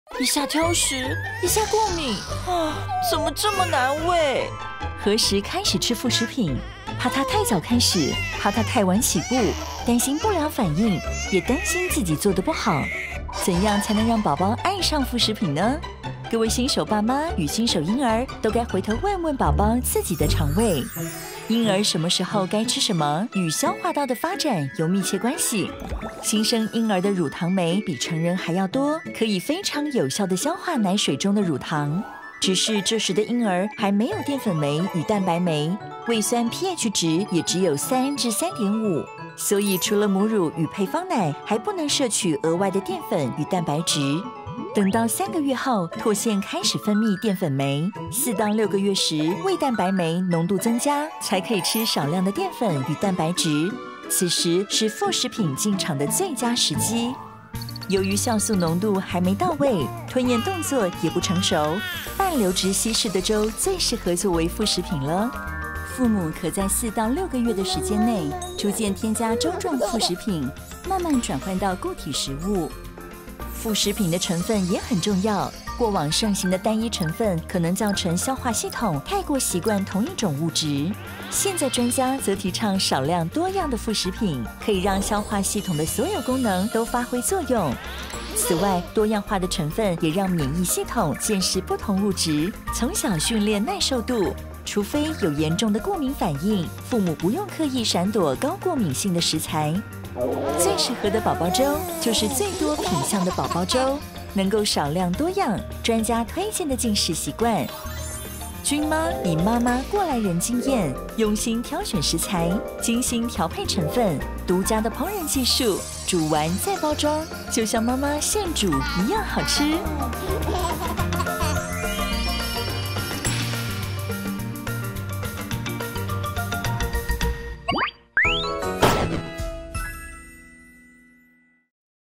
寶寶何時該吃副食品？問天問地問腸胃！
何時該喂幼兒吃副食品，是許多新手爸媽的煩惱，擔心太早起步，害怕太晚進場，或是憂心過敏、挑食。該如何讓「新手嬰兒」愛上副食品？就要回頭瞧瞧他們的腸胃！

原來嬰兒的乳糖酶比成人還要多，要到何時才適合吃副食品？專家建議該怎麼吃，才能讓消化系統發揮功能？「鈞媽寶寶粥」擁有超美味品項，以獨家烹飪技術煮完再包裝，就像媽媽現煮一樣好吃！

※本動畫由 鈞媽御食堂 委託，泛科學企劃執行※

0:00 寶寶超難餵，該問天問腸胃？
0:21 副食品進場時機要看消化道發展
0:57 越吃越有見識訓練幼兒耐受度！
1:40 比媽媽現煮還好吃、還多樣的寶寶粥